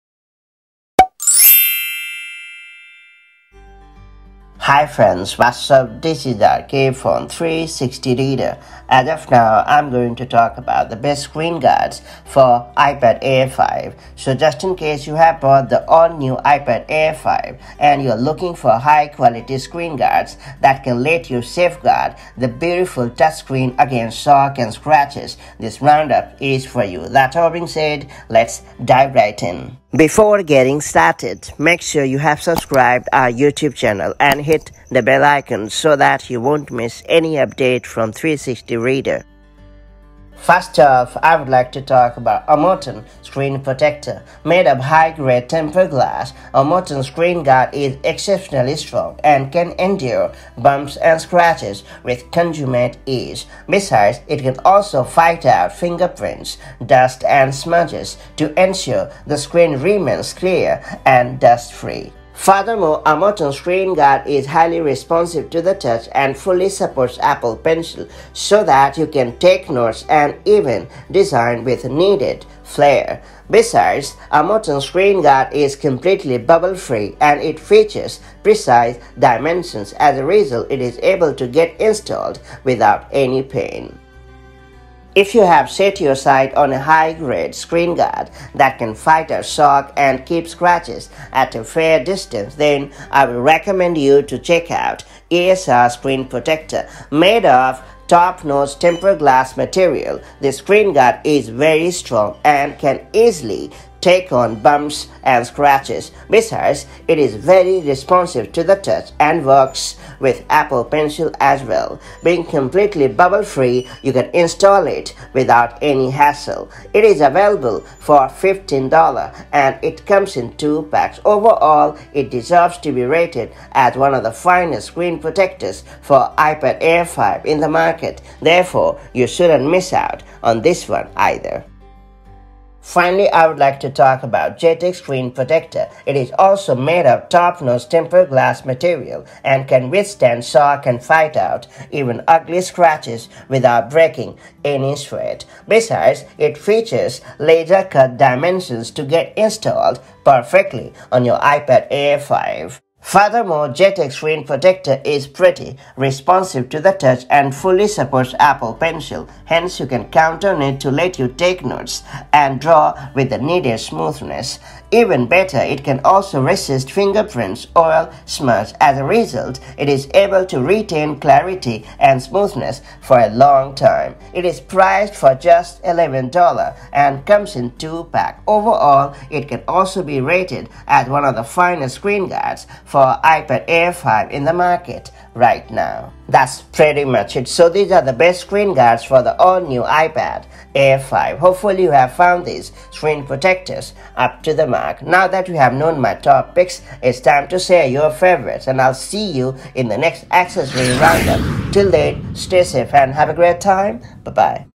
Best iPad Air 5 Screen Protectors You Can Buy in 2022 🔥
Here is the list of the best screen guards for iPad Air 5 you can buy in 2022. Let’s find out!